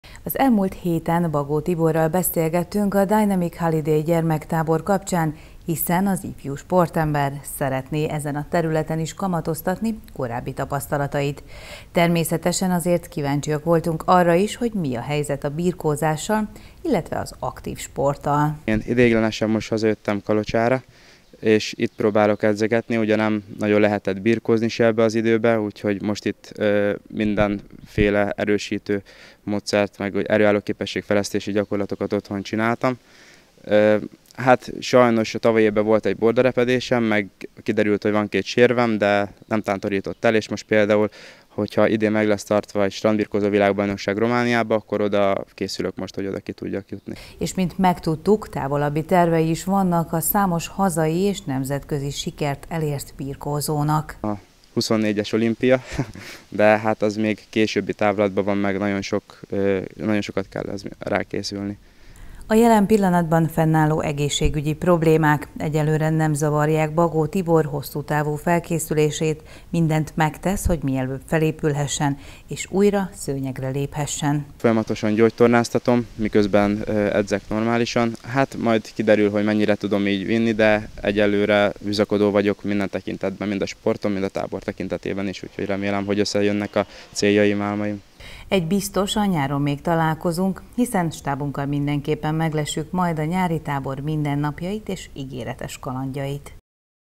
Most itthon van és új kihívásokat vállal - Bagó Tibort jelenéről és a jövőről kérdeztük
Egészségügyi okokból szünetet tart városunk ismert, hazai és nemzetközi szinten is eredményes birkózója. Bagó Tibor most új területen is kipróbálja magát, de mint megtudtuk a sportról sem feledkezik meg.